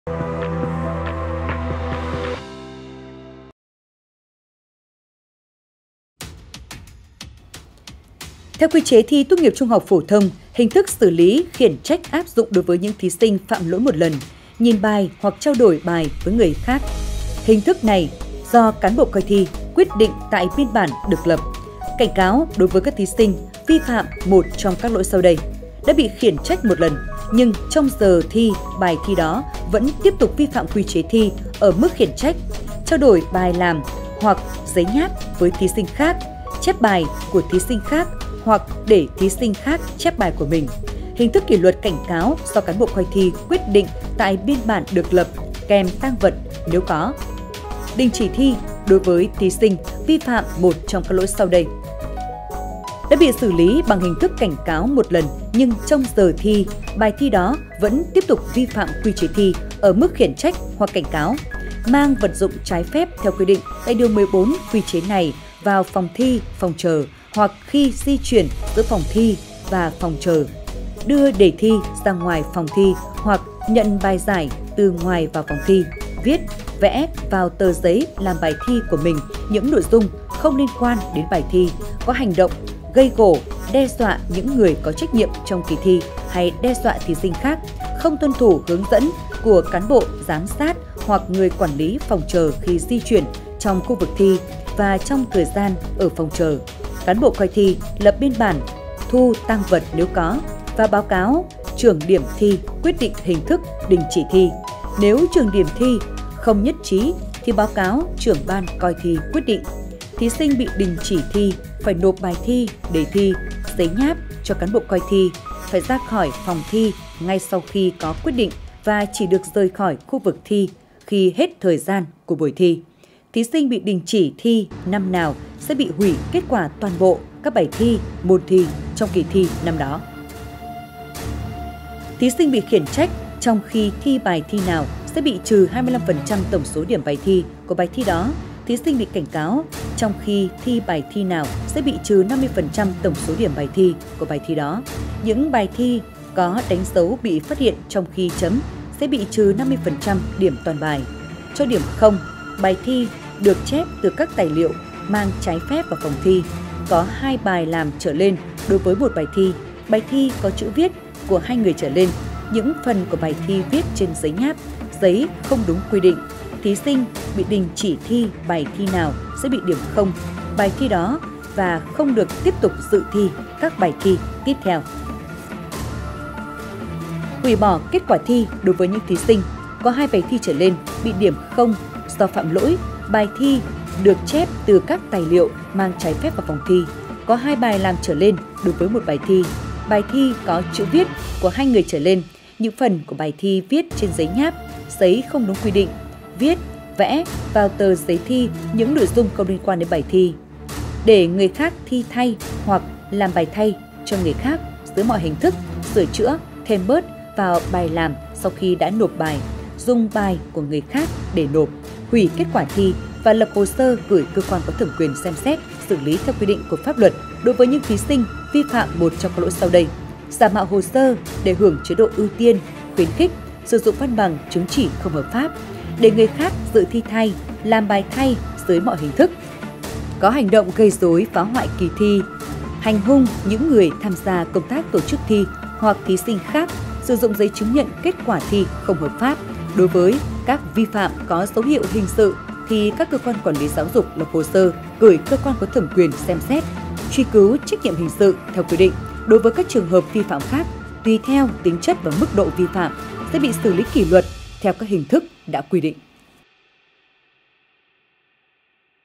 Thí sinh vi phạm Quy chế thi tốt nghiệp THPT sẽ bị xử lý thế nào ?
Hưng Yên TV | Theo Quy chế thi tốt nghiệp THPT, hình thức xử lý khiển trách áp dụng đối với những thí sinh phạm lỗi một lần; nhìn bài hoặc trao đổi bài với thí sinh khác. Hình thức này do cán bộ coi thi quyết định tại biên bản được lập. Cảnh cáo đối với các thí sinh vi phạm một trong các lỗi sau đây: Đã bị khiển trách một lần nhưng trong giờ thi bài thi đó vẫn tiếp tục vi phạm Quy chế thi ở mức khiển trách; trao đổi bài làm hoặc giấy nháp với thí sinh khác; chép bài của thí sinh khác hoặc để thí sinh khác chép bài của mình. Hình thức kỷ luật cảnh cáo do cán bộ coi thi quyết định tại biên bản được lập, kèm tang vật (nếu có). Đình chỉ thi đối với các thí sinh vi phạm một trong các lỗi sau đây: Đã bị xử lý bằng hình thức cảnh cáo một lần nhưng trong giờ thi bài thi đó vẫn tiếp tục vi phạm quy chế thi ở mức khiển trách hoặc cảnh cáo; Mang vật dụng trái phép theo quy định tại Điều 14 Quy chế này vào phòng thi/phòng chờ hoặc khi di chuyển giữa phòng thi và phòng chờ; đưa đề thi ra ngoài phòng thi hoặc nhận bài giải từ ngoài vào phòng thi; Viết, vẽ vào tờ giấy làm bài thi của mình những nội dung không liên quan đến bài thi; có hành động gây gổ, đe dọa những người có trách nhiệm trong kỳ thi hay đe dọa thí sinh khác; không tuân thủ hướng dẫn của cán bộ giám sát hoặc người quản lý phòng chờ khi di chuyển trong khu vực thi và trong thời gian ở phòng chờ; Cán bộ coi thi lập biên bản, thu tang vật (nếu có) và báo cáo Trưởng Điểm thi quyết định hình thức đình chỉ thi. Nếu Trưởng Điểm thi không nhất trí thì báo cáo Trưởng ban Coi thi quyết định. Thí sinh bị đình chỉ thi phải nộp bài thi, đề thi, giấy nháp cho cán bộ coi thi, phải ra khỏi phòng thi ngay sau khi có quyết định và chỉ được rời khỏi khu vực thi khi hết thời gian của buổi thi; Thí sinh bị đình chỉ thi năm nào sẽ bị hủy kết quả toàn bộ các bài thi/môn thi trong kỳ thi năm đó.
Địa chỉ: 164 Nguyễn Văn Linh, thành phố Hưng Yên, tỉnh Hưng Yên
Tiktok: Truyền hình Hưng Yên
Facebook: Đài Phát thanh và Truyền hình Hưng Yên
Zalo: Đài PTTH Hưng Yên